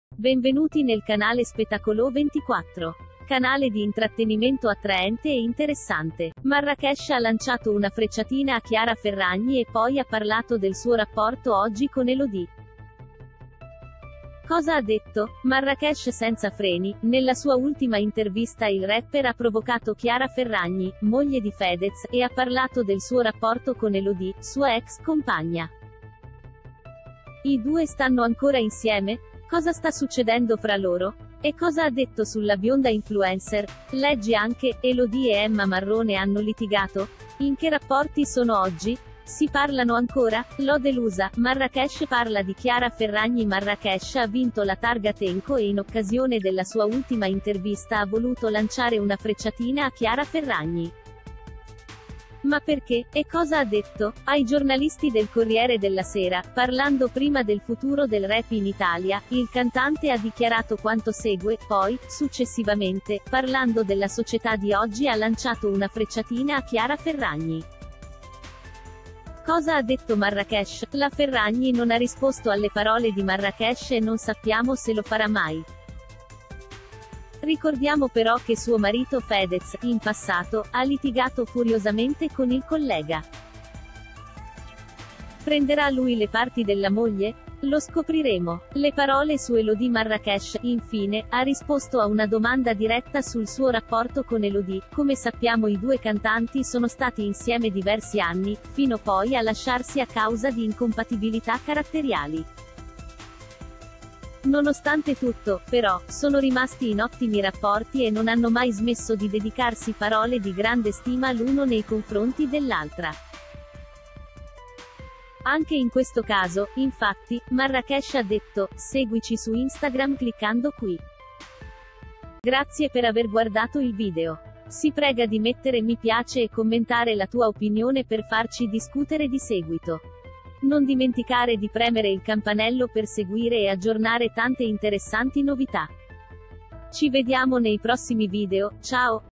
Marracash contro Chiara Ferragni: «Ingiustizia sociale», poi parla di Elodie, stanno ancora insieme?
Marracash ha lanciato una frecciatina a Chiara Ferragni e poi ha parlato del suo rapporto oggi con Elodie. Cosa ha detto?   Marracash senza freni: nella sua ultima intervista il rapper ha provocato Chiara Ferragni, moglie di Fedez, e ha parlato del suo rapporto con Elodie, sua ex compagna. I due st